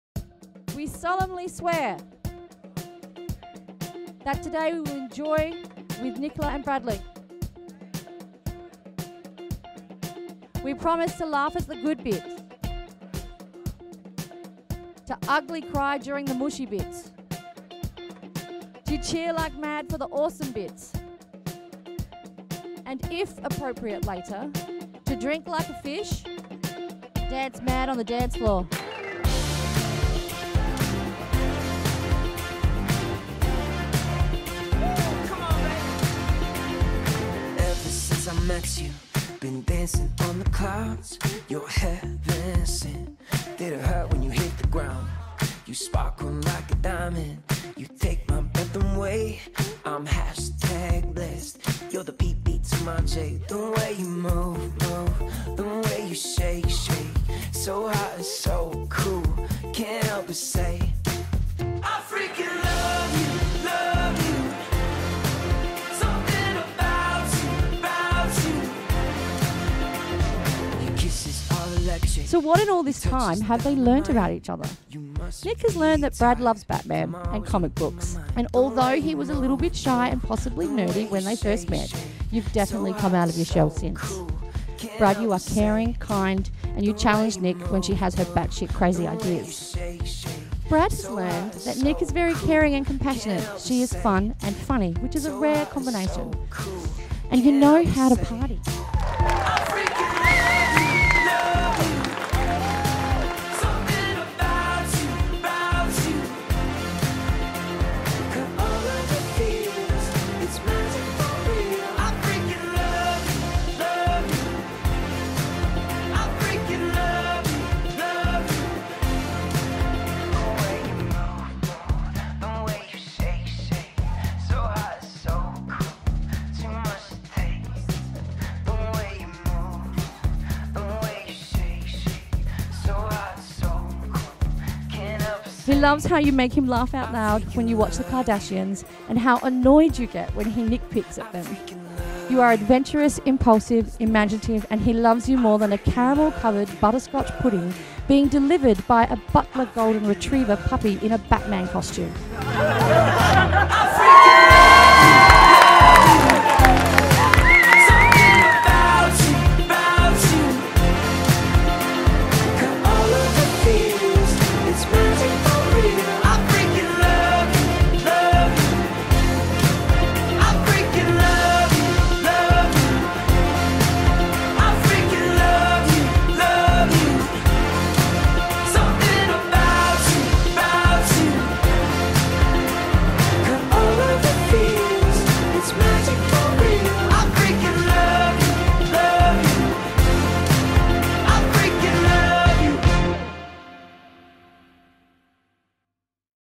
Legendary Couple Let LOOSE on the Dancefloor | Barossa Valley Winery Wedding Party!
Nik and Brad are two madly in love, super fun, dead-set legends! Surrounded by their favourite people (AKA their ‘Wolf Pack’) and a bunch of incredible family and friends, these guys pulled off a classy ‘do’ in the best place ever, the Barossa Valley. I say classy, and I mean it, but as the sun went down, these guys certainly let loose on the dance floor and being part of it was something else!

These two have the sweetest relationship filled to the brim with cheek, jokes and laughs. Their ceremony was one of the funniest I have witnessed and from the get go they were just plain fun! But it is simply so, so good to get to know genuinely down to earth, good people who you’ll never forget.

We will welcome this gang from Mildura back anytime.

MB01HQKZDXRANTK